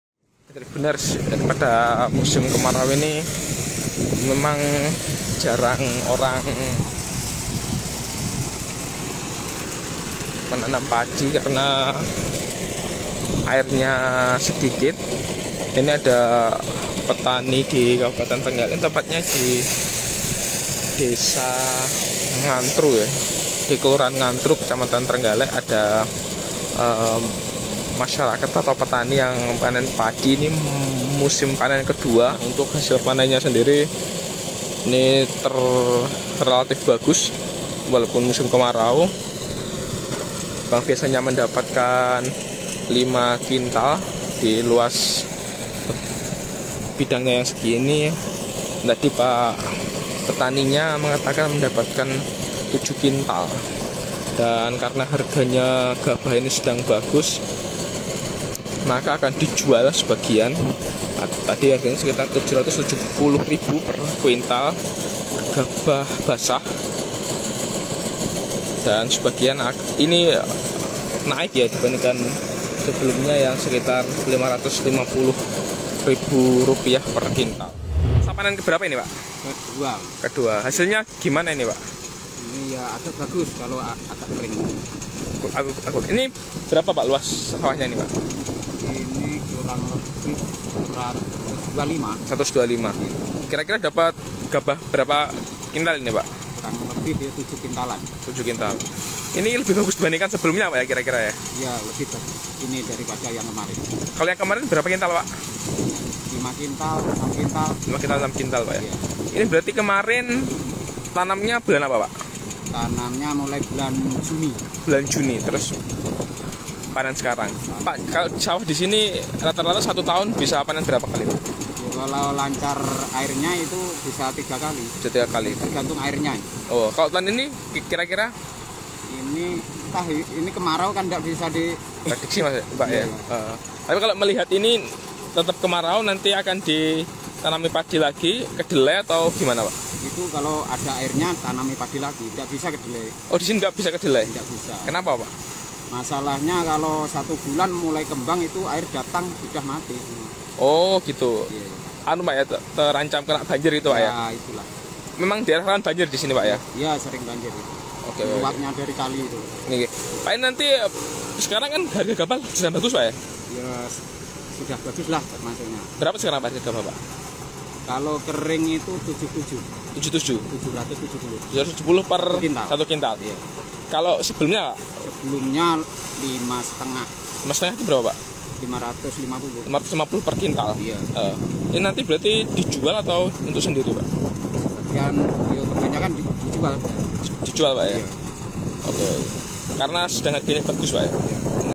Harga Sedang Tinggi, Petani Trenggalek Jual Gabah Hasil Panennya
vp: didit | reporter: sofyan arif candra

TRIBUNMATARAMAN.COM | TRENGGALEK - Sejumlah petani di areal persawahan Kelurahan Kelutan, Kecamatan/Kabupaten Trenggalek mulai memasuki masa panen padi, Kamis (5/10/2023).

Dari puluhan hektar sawah padi yang telah menguning Wigiono menjadi salah satu petani yang paling awal memanen padinya.

"Ini masa panen kedua, Alhamdulillah hasilnya lebih bagus dibandingkan masa panen pertama," kata Wigiono, Kamis (5/10/2023).

Menurut Wigiono, salah satu faktor meningkatnya hasil panen padinya karena peralihan cuaca dari musim hujan ke musim kering yang menyebabkan pertumbuhan padi optimal karena matahari yang bersinar sepanjang hari.

"Kalau musim keringnya terjadi menjelang panen malah bagus," lanjutnya.

Dari petak sawahnya seluas 125 meter persegi, Wigiono mampu menghasilkan gabah seberat 7 kuintal, sedangkan pada masa panen pertama ia hanya mendapatkan 5-6 kuintal.

Rencananya, ia akan menjual sebagian hasil panennya tersebut karena harga gabah kering giling sedang bagus.

"Sekarang harganya Rp 770 ribu per kuintal, kalau harga-harga sebelum Rp 550 ribu per kuintal," tambahnya.

Lebih lanjut, Wigiono belum memutuskan apakah akan menanam padi lagi pada musim tanam ketiga atau mengosongkan lahannya.

"Kalau ada air saya tanam lagi, tapi kalau masih kering seperti sekarang ya tidak ditanami," ucap Wigiono.

"Soalnya kalau saya tanami kedelai, baru berumur dua bulan masuk musim hujan bisa kebanjiran malah gagal panen, di sini rawan kebanjiran luapan air Sungai Ngasinan," pungkasnya.